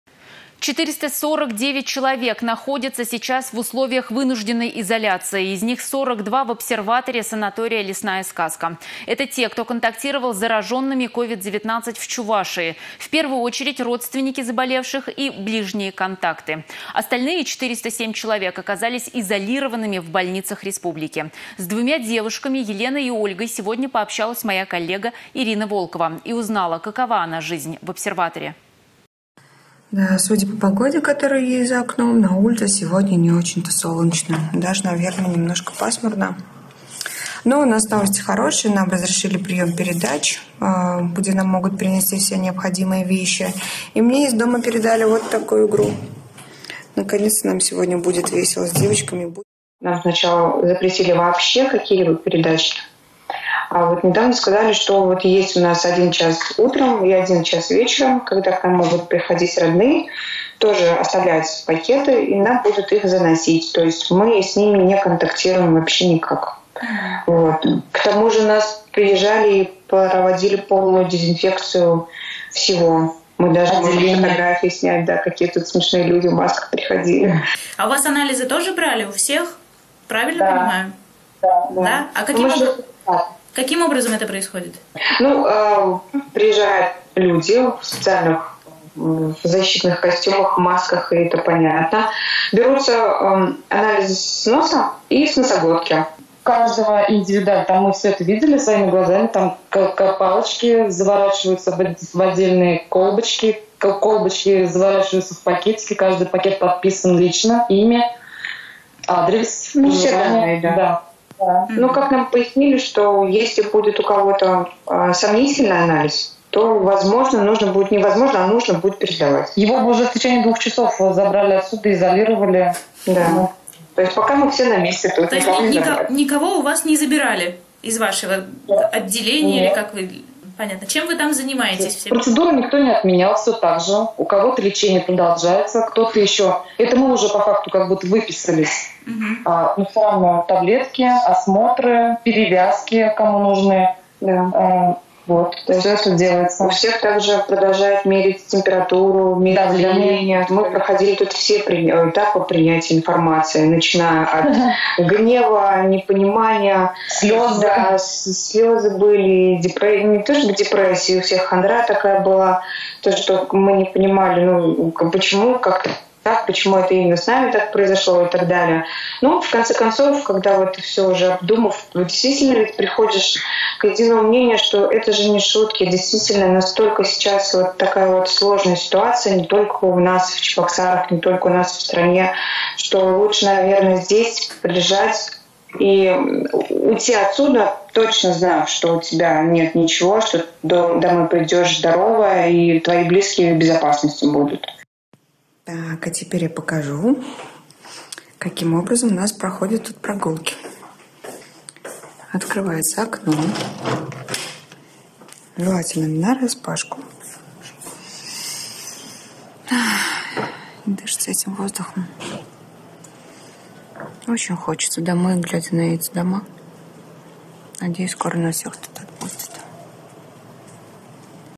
Жизнь в обсерваторе
449 человек находятся сейчас в условиях вынужденной изоляции. Из них 42 в обсерваторе санатория «Лесная Сказка». Это те, кто контактировал с зараженными covid-19 в Чувашии, в первую очередь родственники заболевших и ближние «контакты». Остальные — 407 человек оказались изолированными в больницах республики. С двумя девушками Еленой и Ольгой сегодня пообщалась моя коллега Ирина Волкова и узнала, какова она — жизнь в обсерваторе.